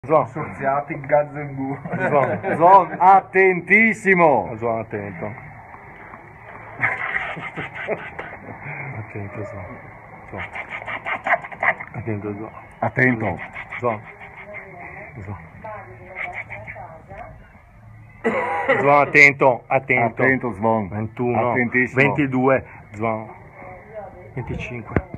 Atento Zvon ep.64 - Massimo & Guitar reachin' Stonesland
Recente incontro a Stonesland in occasione di una reunion Stonesiana. The Original non mancano l'occasione per ricordare a Zvon di stare sempre Atento!